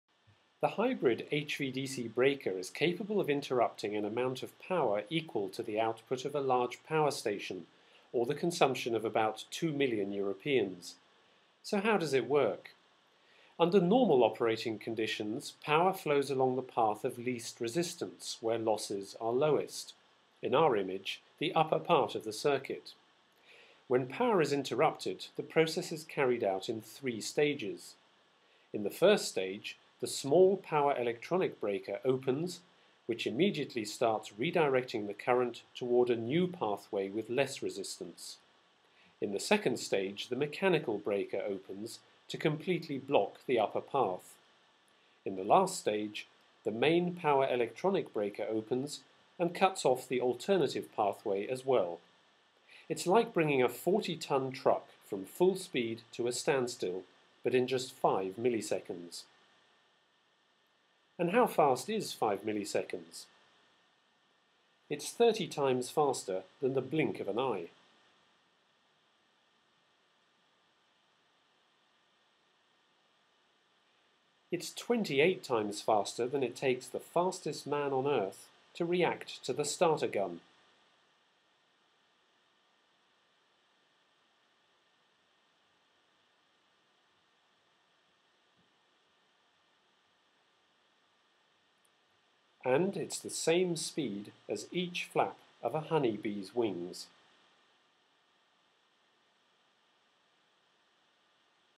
Hybrid HVDC Breaker, how does it work?
On November 7, 2012, ABB announced that it solved a central puzzle of electrical engineering. This breakthrough paves the way for the development of the next generation of the electricity grid.

For more info you may to read the following blog post

Additionally, here is a white paper on the hybrid HVDC breaker